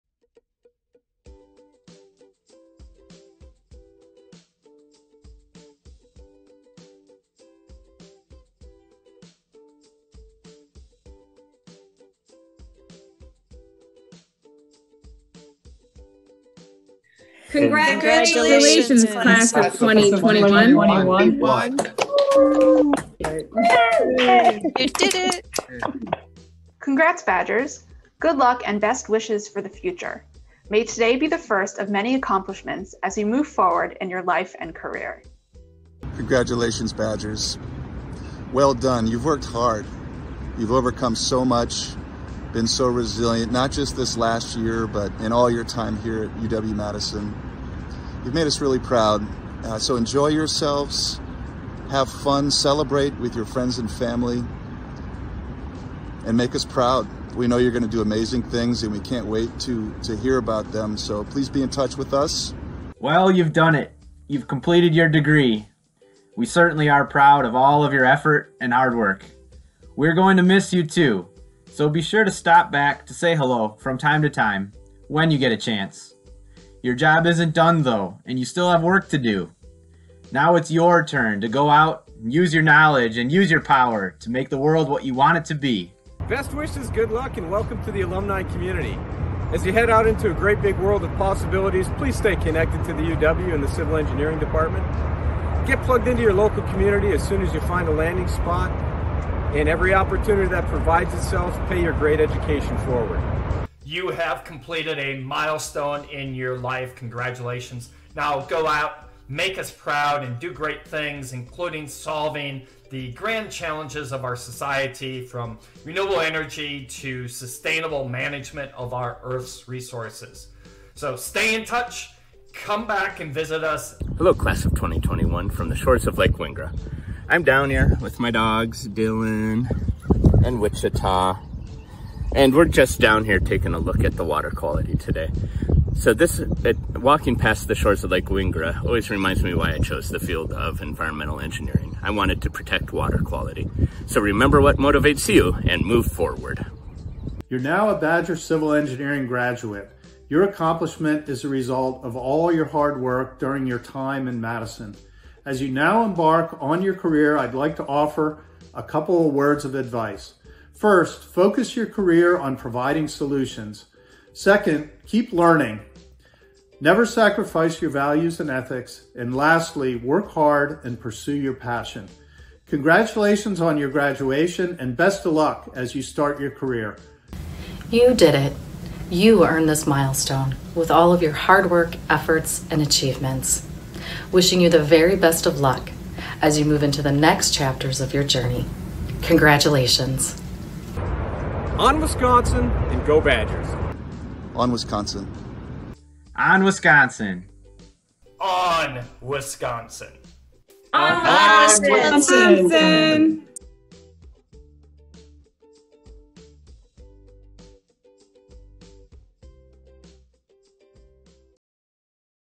Honoring the UW-Madison Civil and Environmental Engineering Class of 2021
Faculty, staff, and alumni of the UW-Madison Civil and Environmental Engineering Department celebrate the 2021 graduating class of Badger Engineers by sharing words of advice and well wishes.